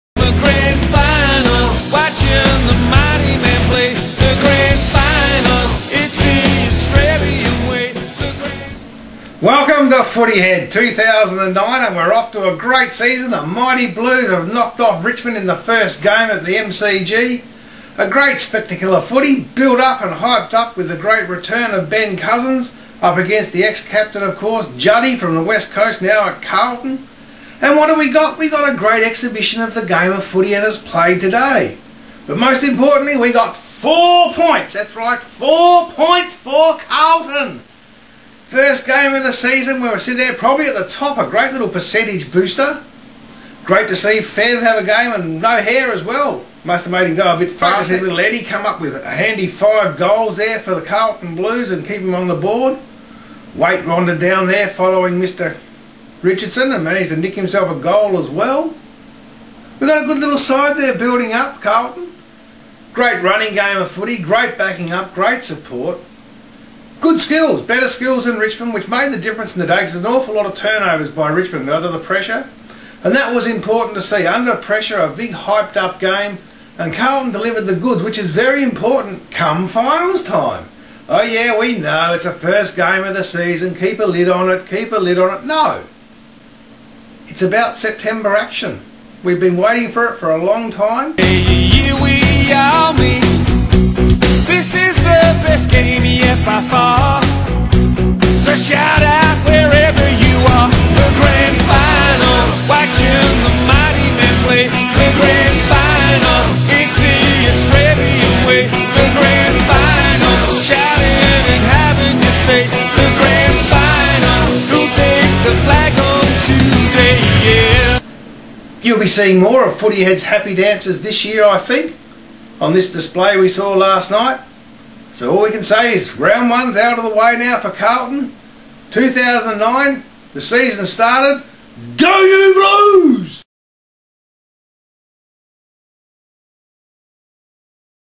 Carlton v Richmond "Footyhead" rd 1 2009
Rd 1 Carlton v Richmond 2009 "Footyhead" post game comments